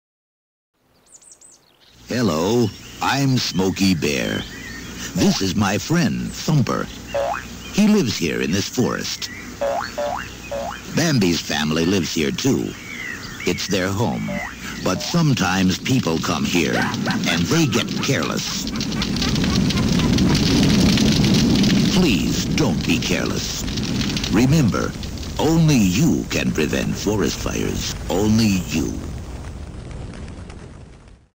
Funny Smokey the Bear Commercial 2011
Smokey the Bear Ads were pretty awesome back in the day and I was wondering why haven't we see him lately? I thought I'd recreate the 1977 radio spot into a low-fi animation for you kids out there to enjoy and help put fear into you at an early age. Humans are destructive and evil to Smokey and his many forest friends so make sure you don't start a forest fire because Smokey will hunt you down!